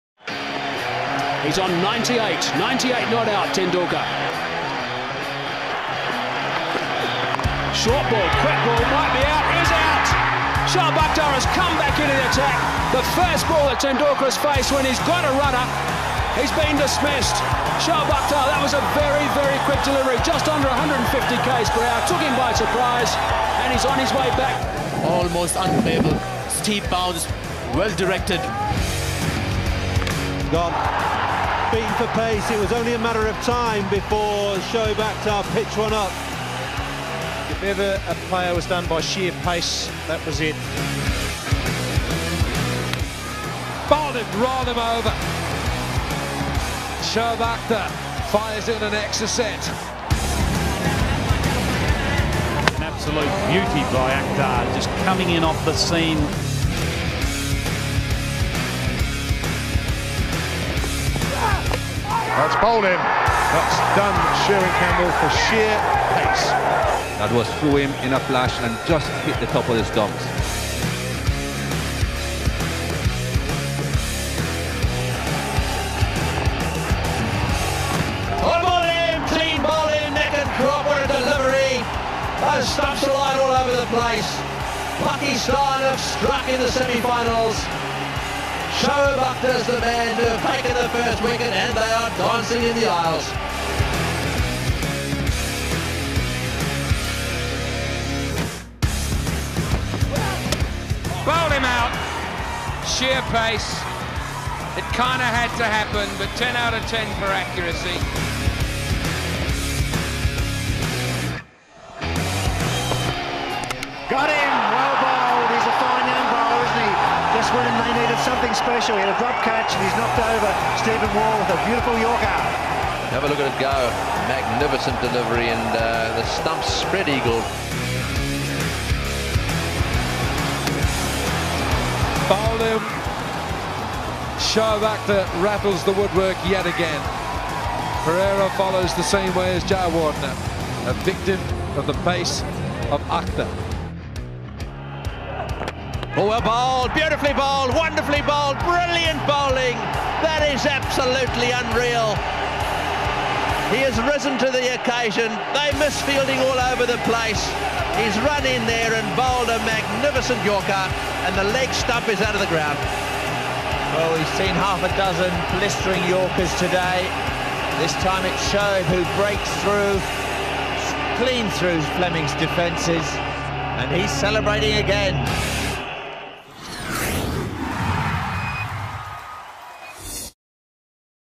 𝐄𝐱𝐩𝐫𝐞𝐬𝐬 𝐩𝐚𝐜𝐞! Celebrate Shoaib Akhtar birthday by watching him go full throttle#cricket#shoaibakhtar
𝐄𝐱𝐩𝐫𝐞𝐬𝐬 𝐩𝐚𝐜𝐞! Celebrate Shoaib Akhtar 's birthday by watching him go full throttle 🚀